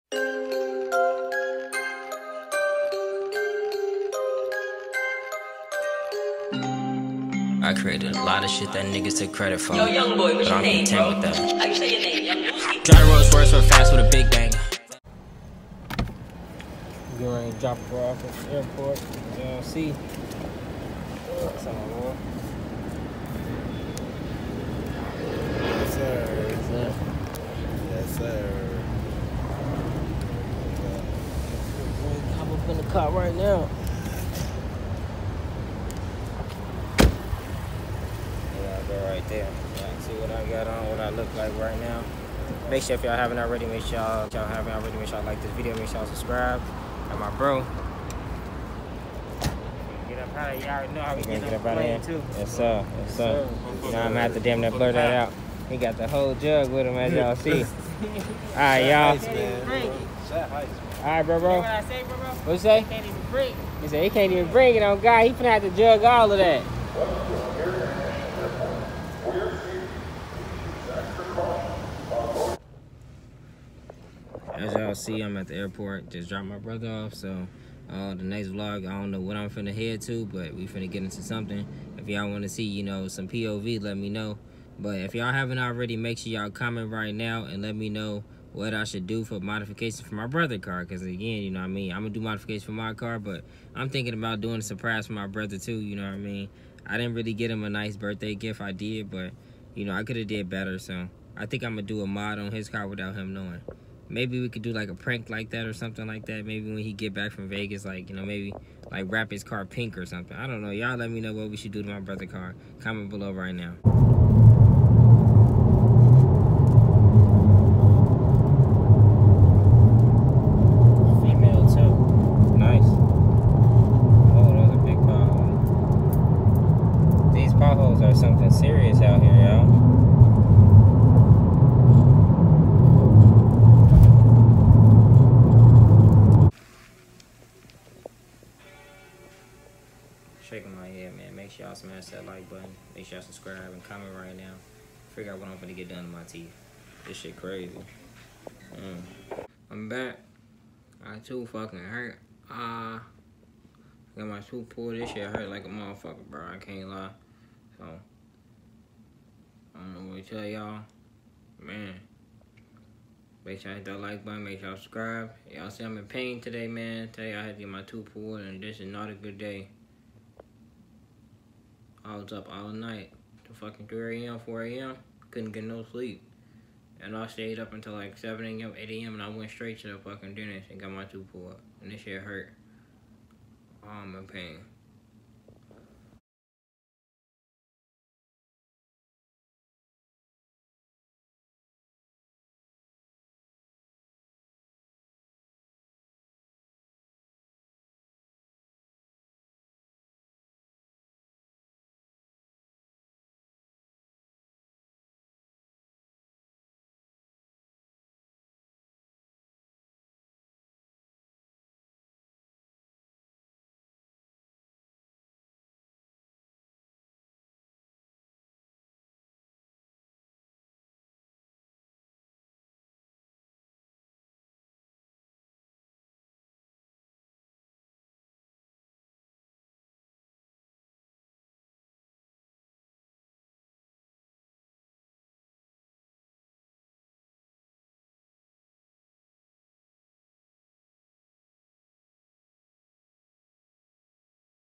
Staying Up 24 HOURS STRAIGHT From A ToothAche! I Dropped Off Roulette King @ChicoTwins The Airport
@ChicoTwins  (MAIN CHANNEL)